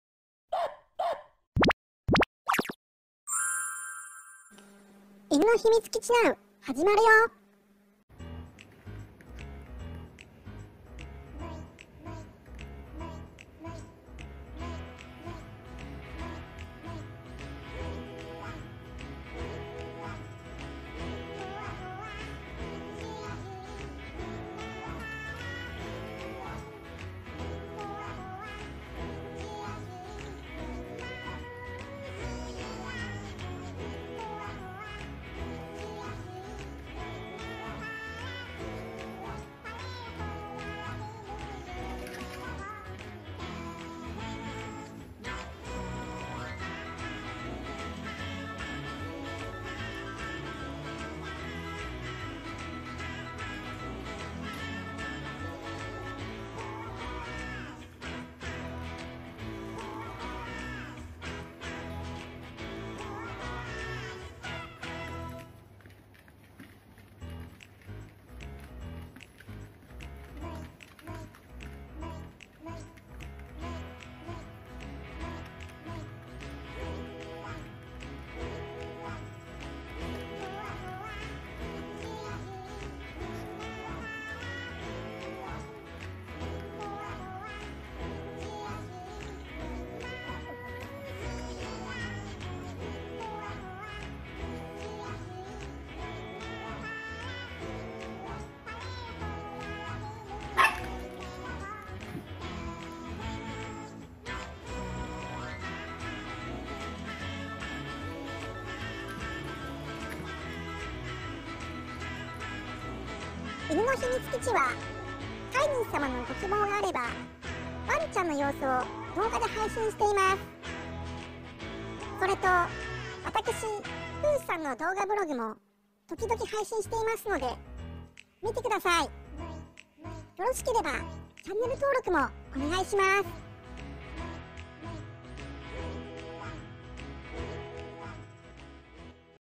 ポメラニアンにグイグイいくビションフリーゼ 🐶ひみつきちなう！#1🐶 (チワワ ビションフリーゼ ポメラニアン ミックス犬 マルチーズ ウェスティ) 2021年2月27日
こまちちゃん『グイグイいくーぜ』

ミックス犬　こまめちゃん
ポメラニアン　むくちゃん
ウェスティ　バボリくん